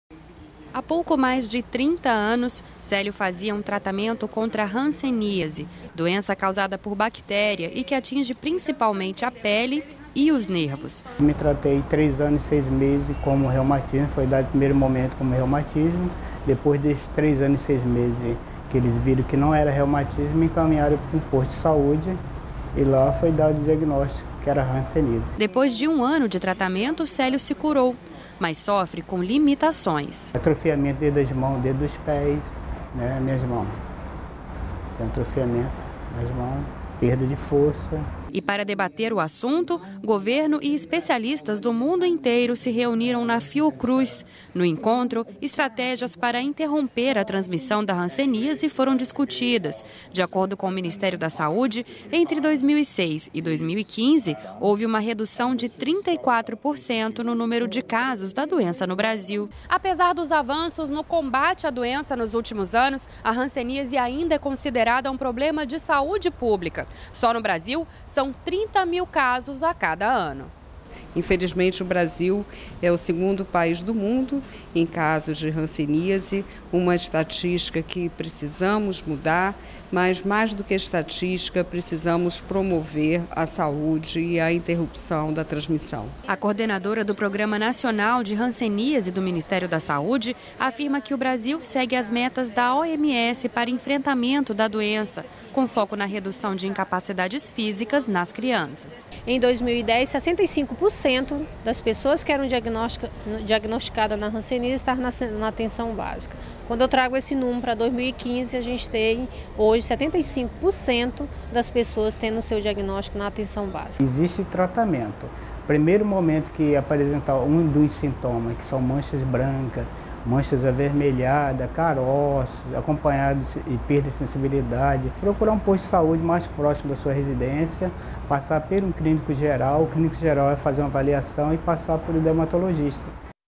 Especialistas se reunem para definir estratégias de combate à hanseníase
REPÓRTER NBR - 31.05.17: Número de casos de hanseníase no Brasil caiu entre 2006 e 2015. A doença ainda é considerada um problema de saúde pública, mas especialistas estão reunidos no Rio de Janeiro para definir estratégias de combate à hanseníase.